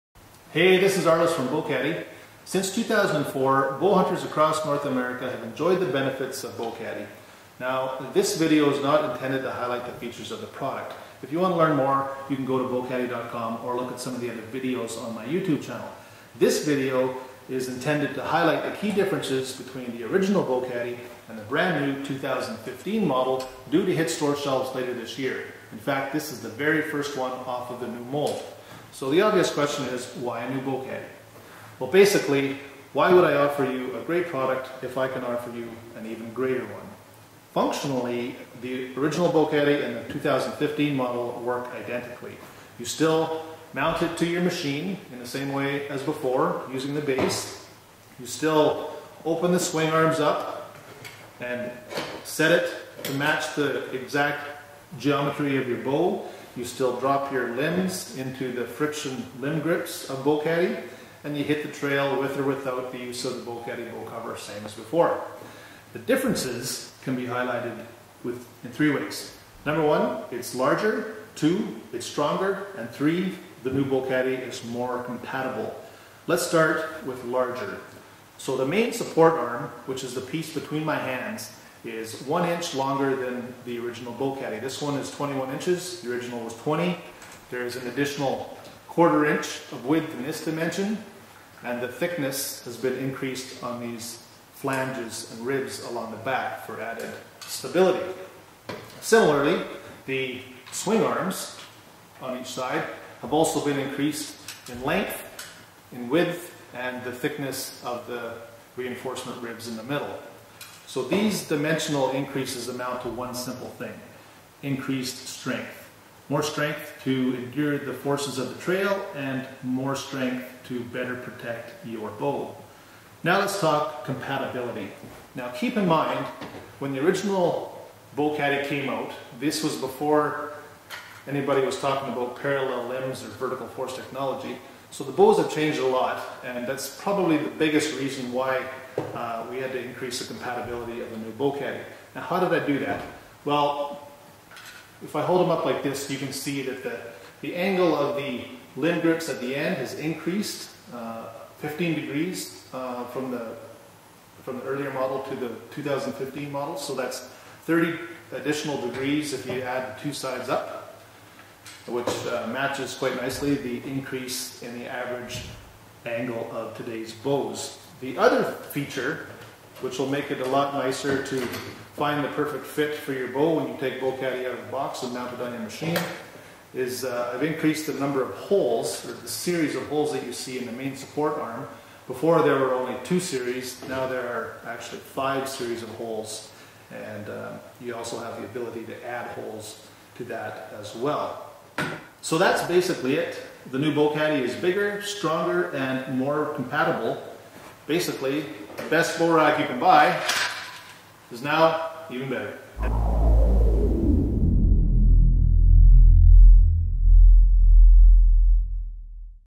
What's Different About the 2015 Bowkaddy Model?
I created this video clip to highlight the important design improvements in the first major update to the Bowkaddy product in over a decade.  Boasting all the same functionality and features of the original Bowkaddy, the new model is dimensionally enhanced, much stronger and more compatible with the parallel limb bows of today.  Who says you can't beat an original!